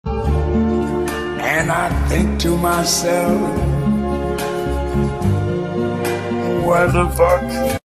wHaT tHe Fu- (turns out my hair grows fast)
Good news is I look more male passing now :]
My pronouns: He/It/They -
Characters
•Brown Hair(me irl): online name, Bug - pronouns, He/It/They